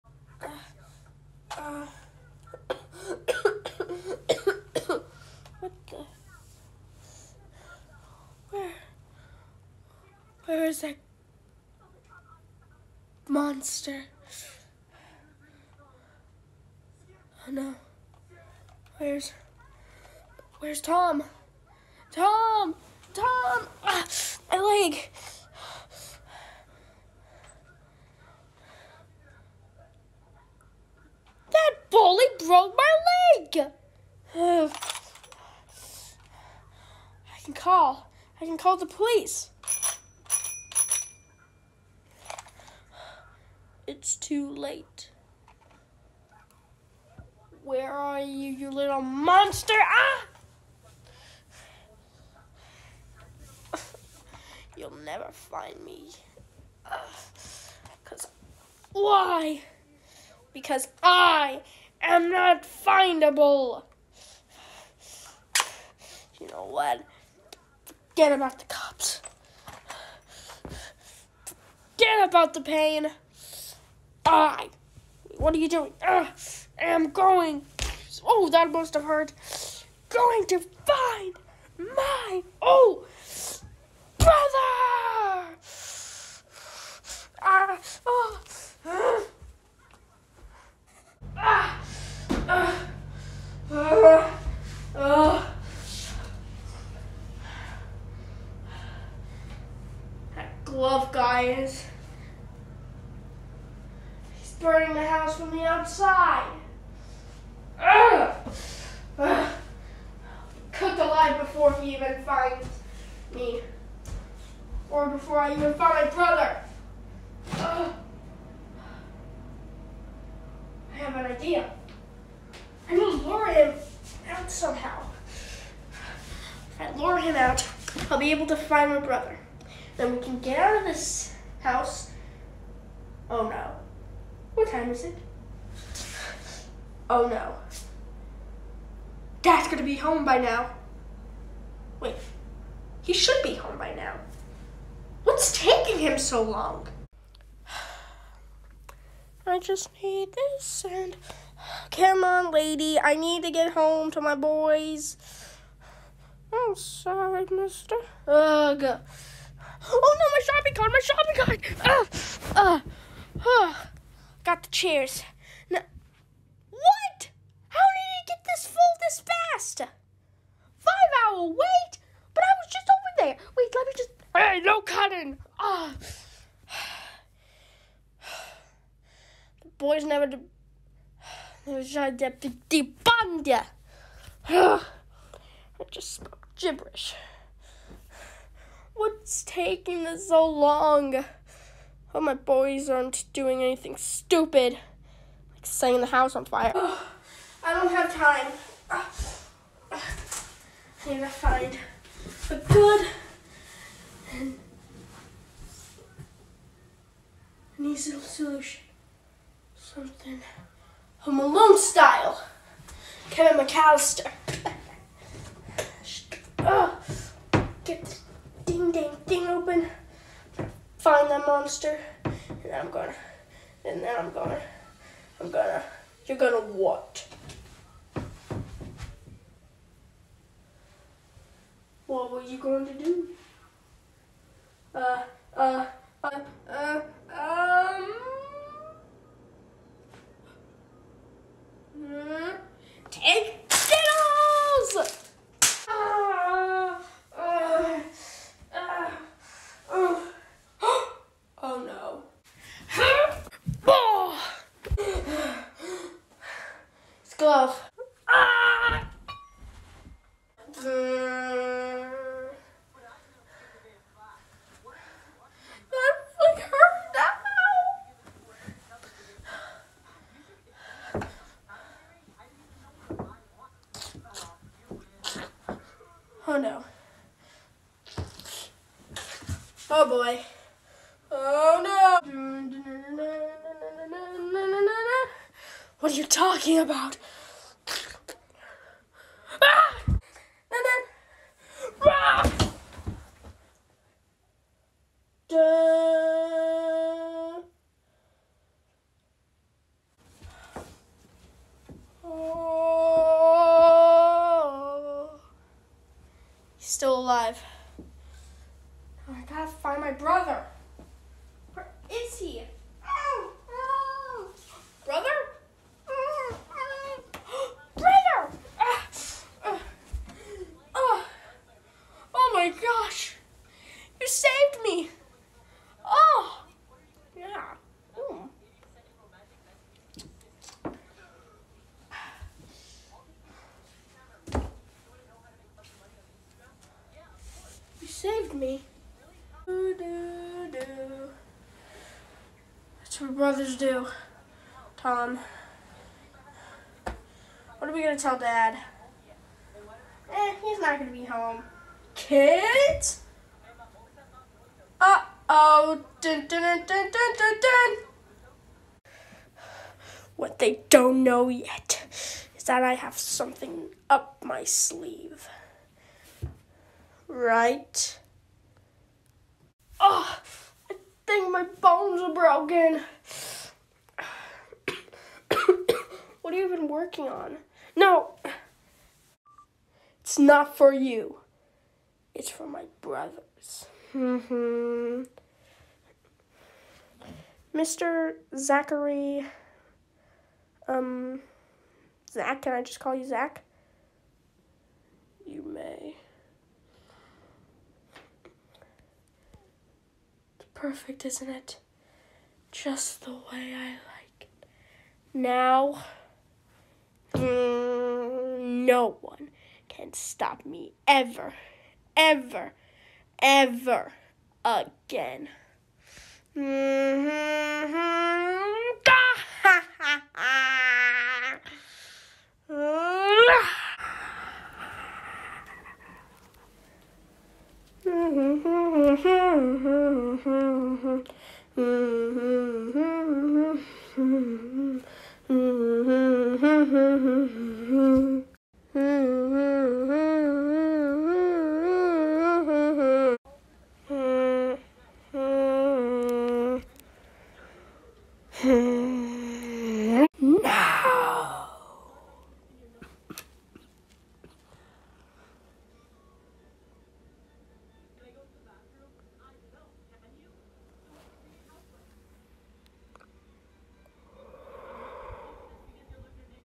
The Thompsons. Episode 2: the (pilot) part 2 of 2
Luke try’s his best to save his brother. While also battling the glove guy